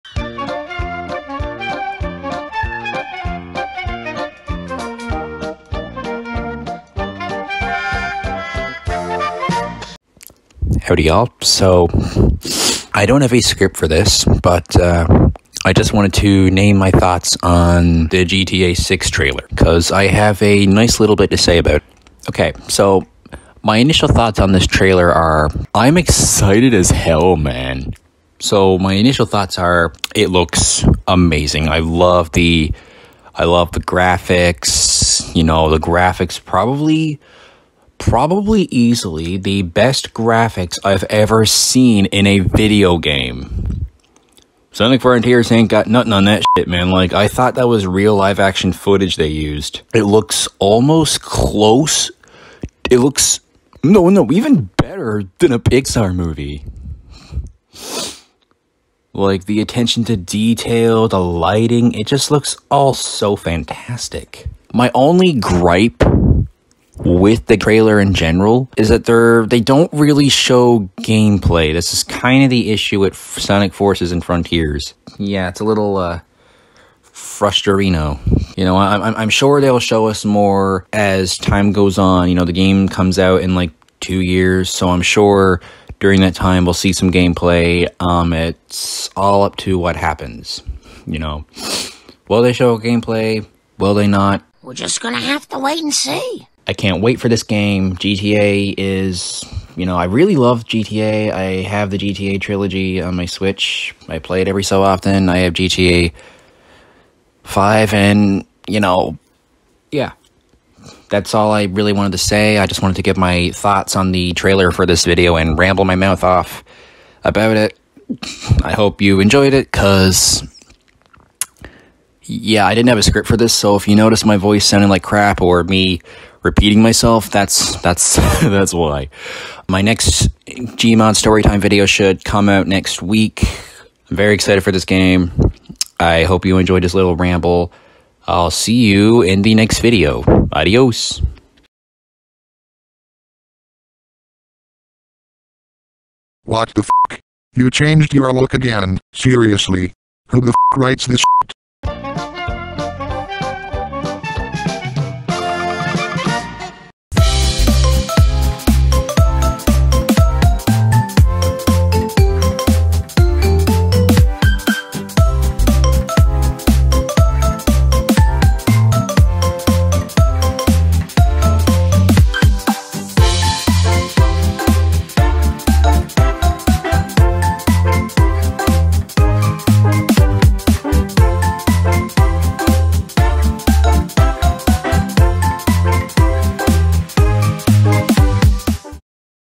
My Thoughts on The Trailer For GTA 6
WARNING: This is NOT a GMOD Storytime, This is just a li'l unscripted ramble video I made out of the blue.